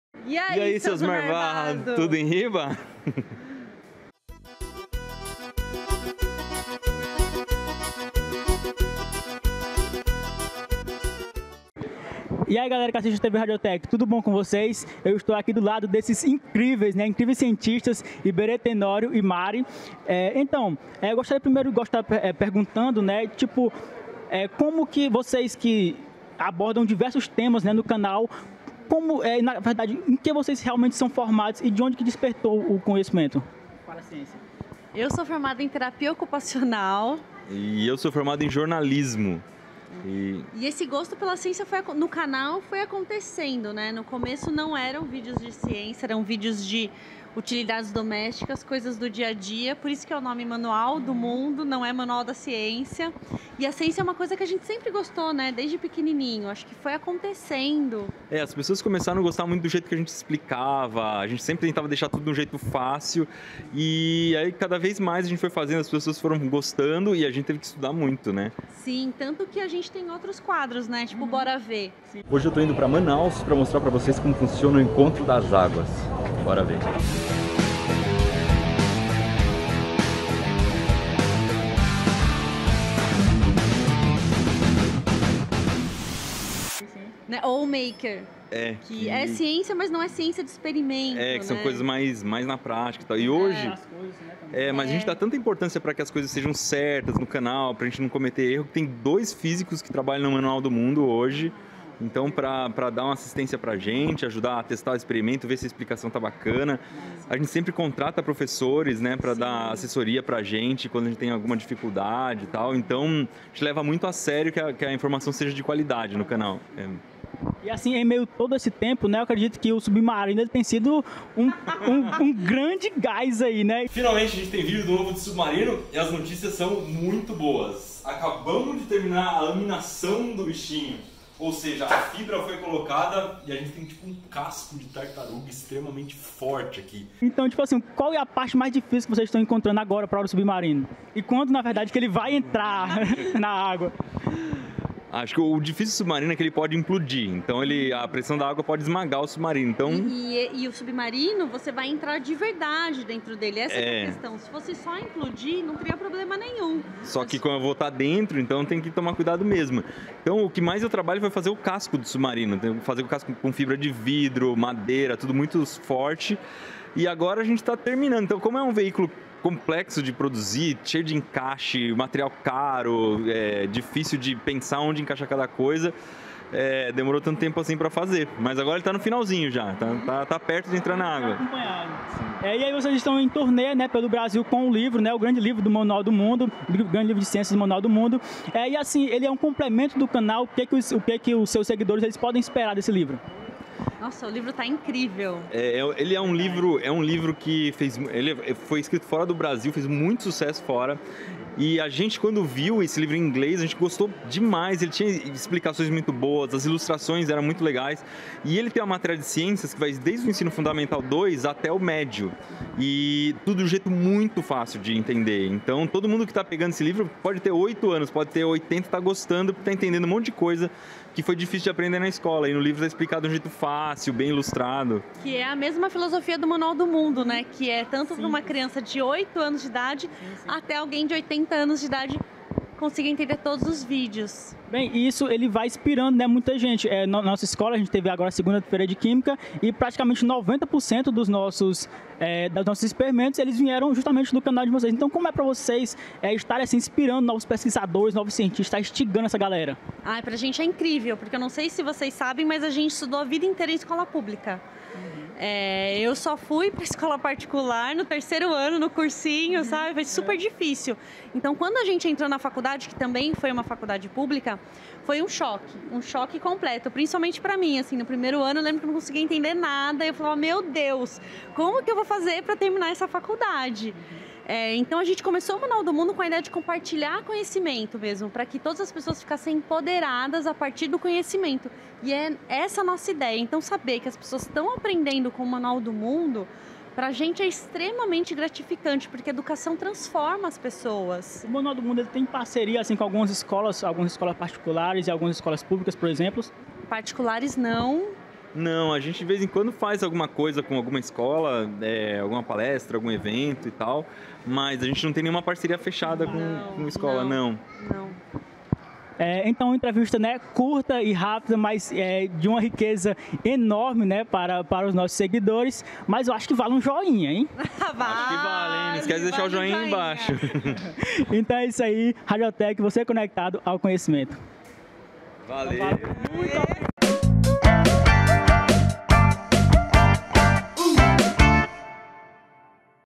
"Compartilhamos conhecimento para as pessoas ficarem empoderadas" - Papo Arretado (62)
No dia 23 de novembro de 2019, Iberê Thenório e Mariana Fulfaro, do Canal Manual do Mundo, estiveram na Livraria Leitura, em Teresina/PI, para o lançamento do livro "O grande livro de Ciências do Manual do Mundo", publicado pela Editora Sextante. A TV Radiotec acompanhou o lançamento e entrevistou Iberê e a Mari. Para essa missão, convidamos Diego Rocha, ex-bolsista, ex-aluno do Colégio Técnico de Floriano e fã do canal, para realizar a entrevista.

Trechos usados na edição do programa foram retirados do Canal do YouTube Manual do Mundo.

Registrar nosso agradecimento especial a Natália Alexandre que viabilizou a participação do Laboratório de Leitura e Produção Textual no lançamento do livro e, especialmente, para que a TV Radiotec realizasse a gravação da entrevista.

Âncora: Diego Rocha
Gravação: Lucas Mariel
Edição: Jarod Mateus

TV RADIOTEC
Você conectad@ ao conhecimento

LABORATÓRIO DE LEITURA E PRODUÇÃO TEXTUAL
Coordenação: Prof. Ribamar Jr